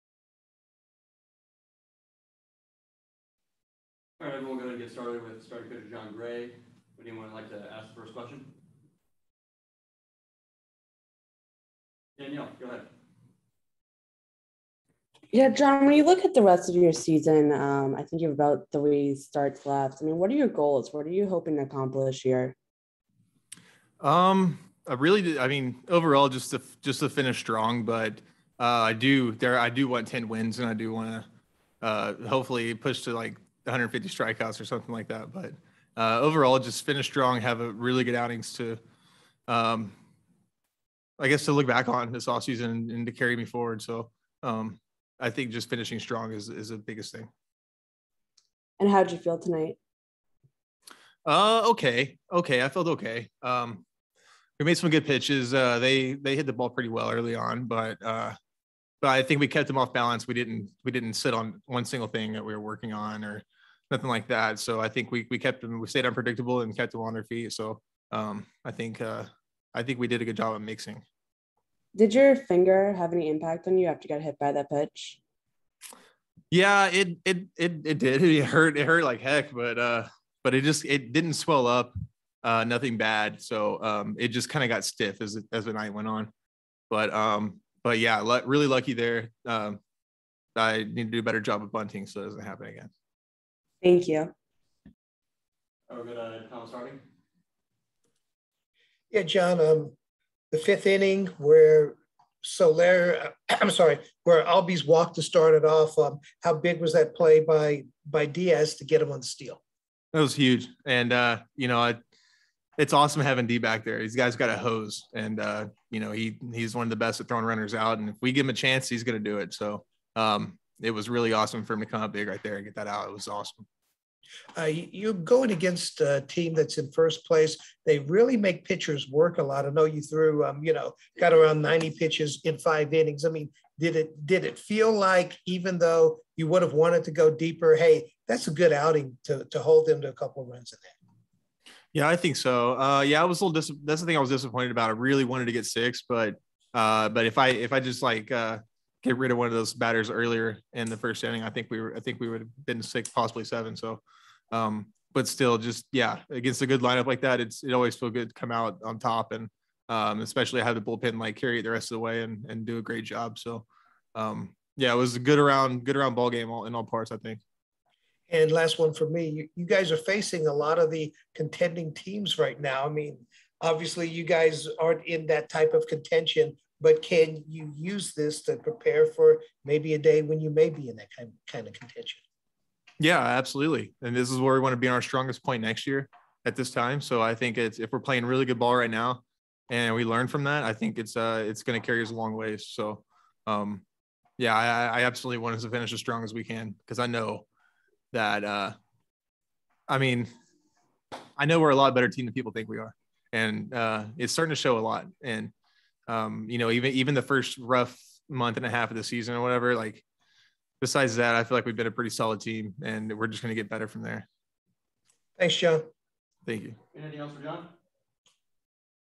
Jon Gray sees final days of season as chance for Rockies to make a statement for 2022
Jon Gray is looking forward to final games of season. Focus is on winning 10 games and finishing with 150 strikeouts. ... Feels strong finish will reinforce that Rockies are a better team than outsiders might think. ... Gray is impressed with emergence of catcher Elias Diaz, who is showing off a strong arm when runners attempt to steal.